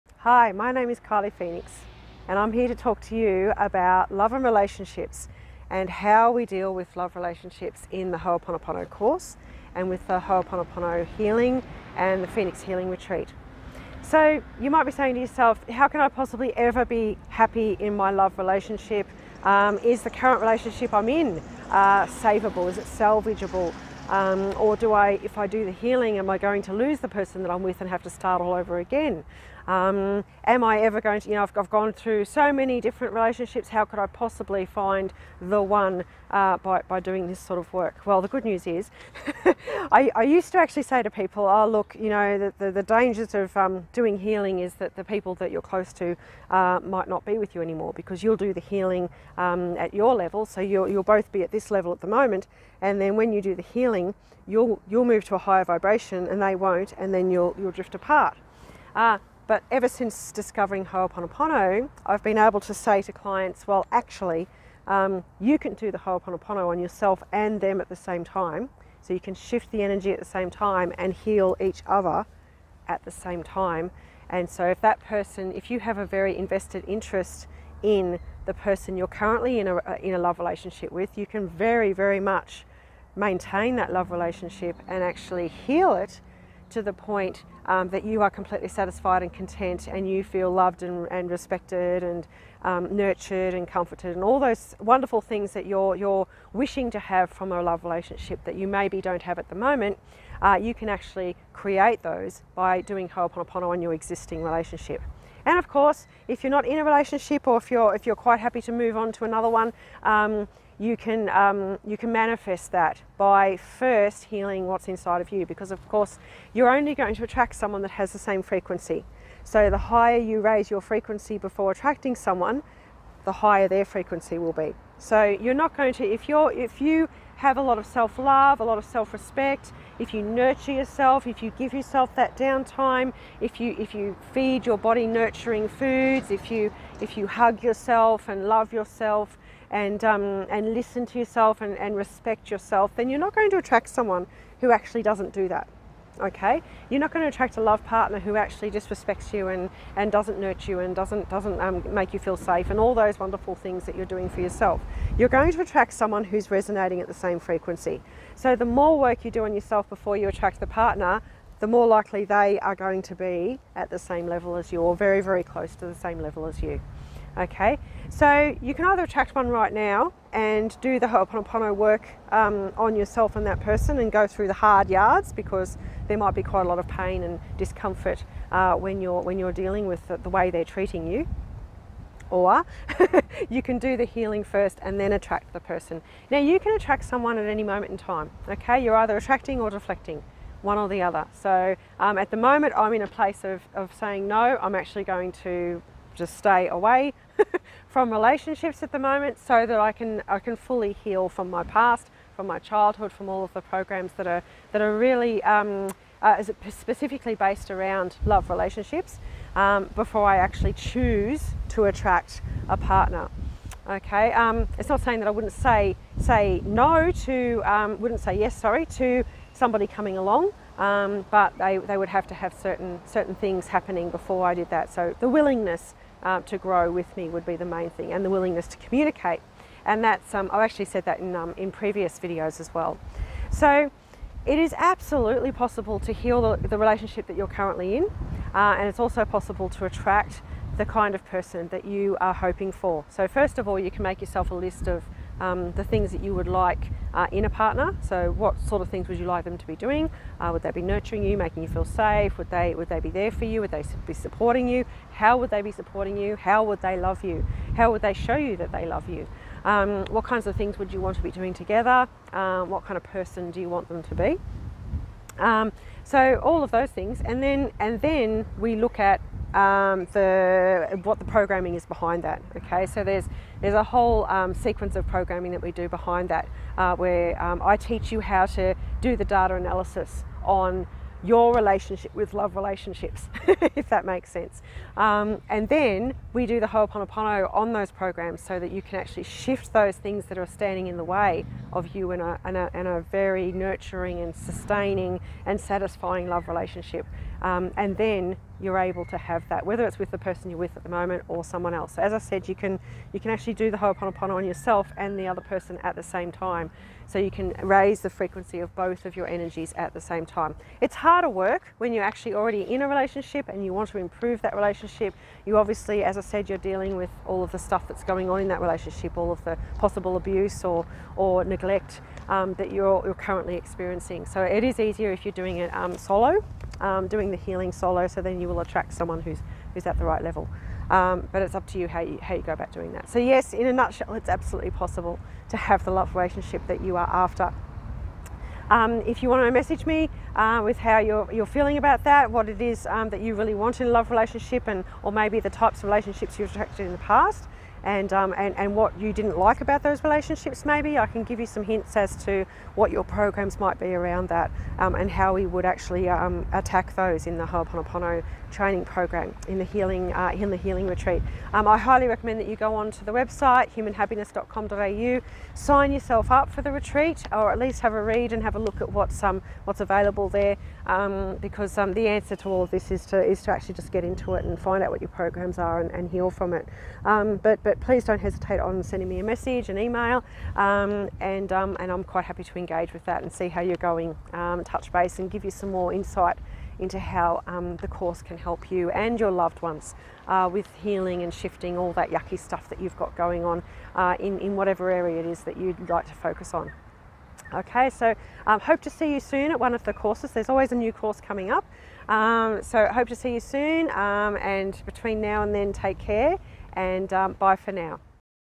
Love Relationships How to attract, sustain and flourish in Healing Retreat
Kylie explains how anyone can attract a love relationship at any moment, but attracting a healthy, sustainable and satisfying one is all about the work you do on yourself before you do the attracting, which can be done by utilising the tools in the Phoenix Healing Retreat.